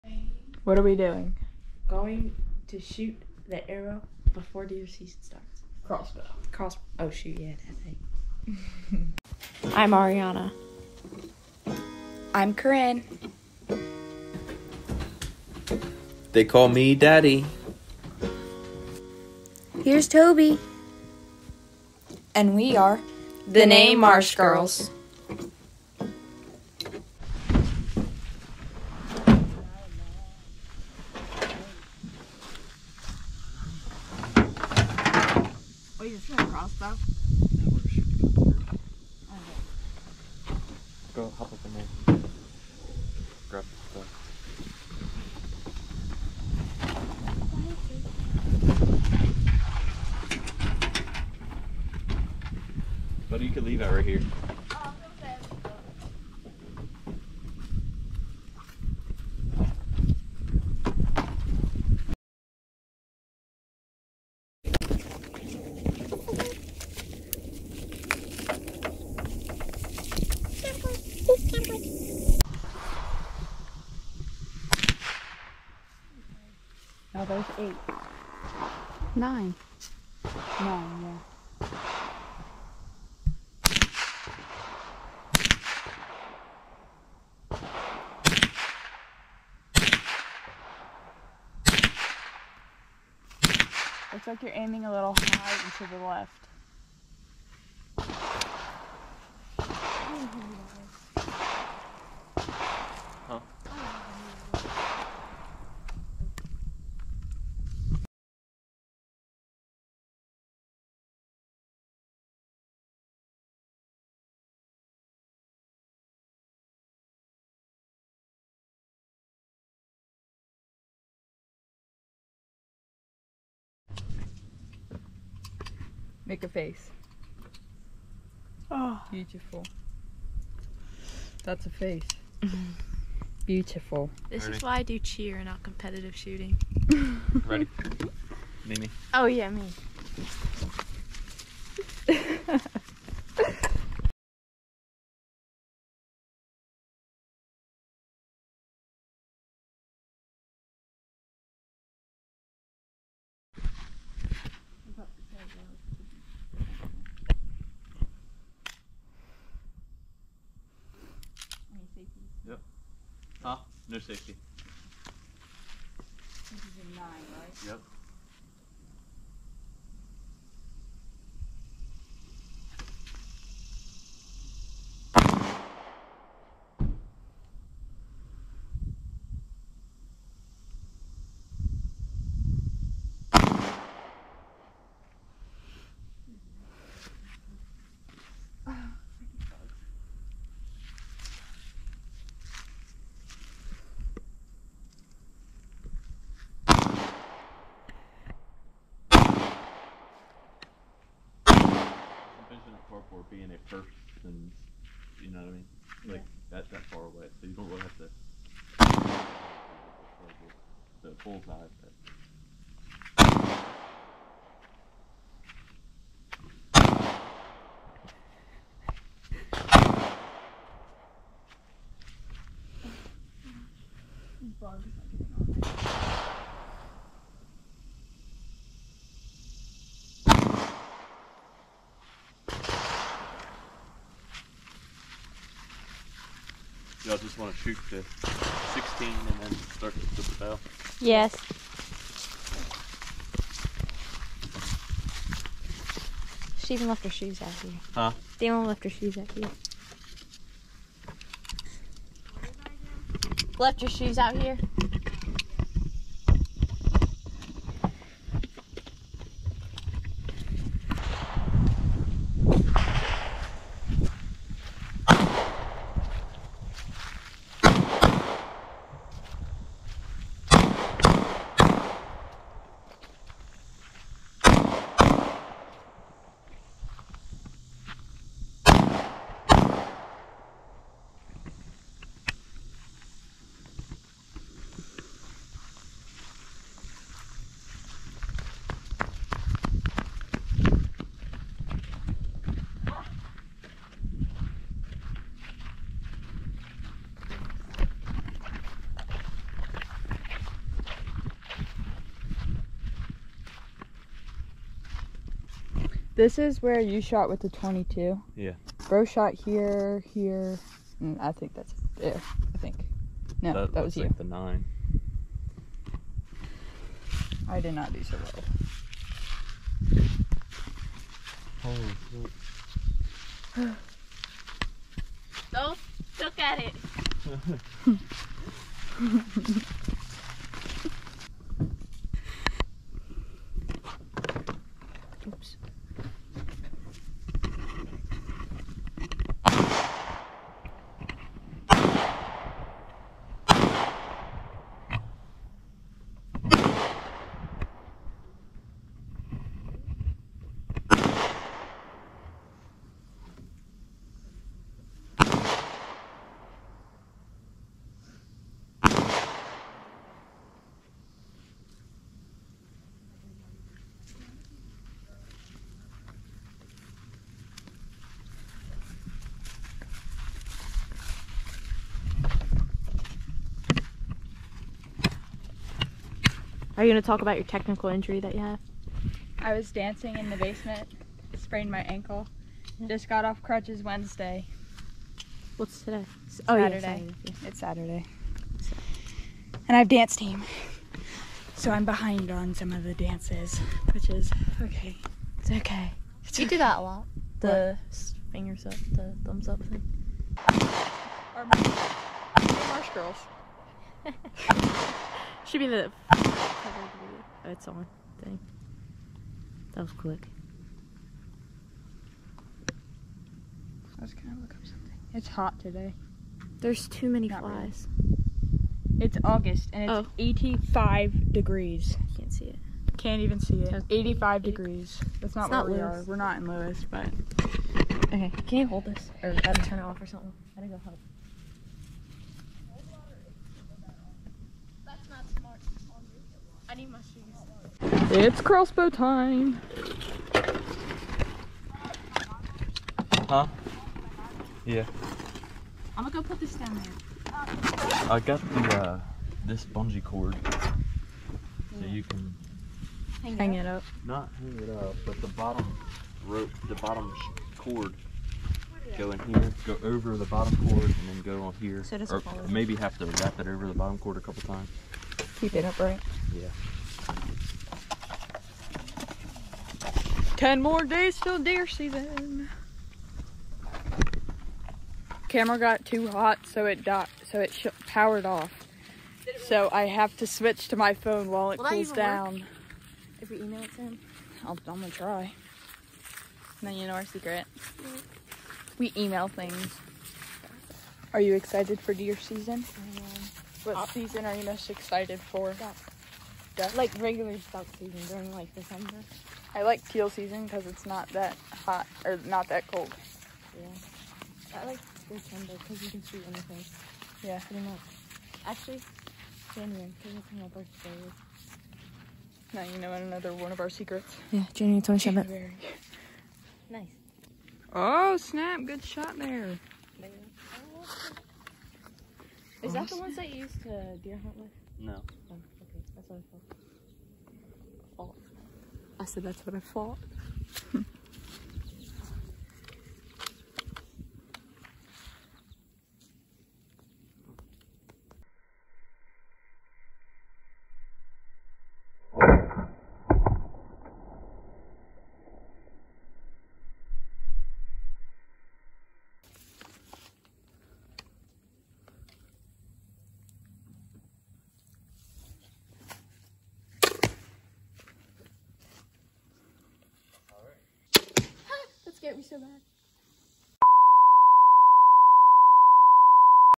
Shooting the Crossbow For Deer Season
Deer season is coming up soon so we went to shoot the crossbow to get ready enjoy the video!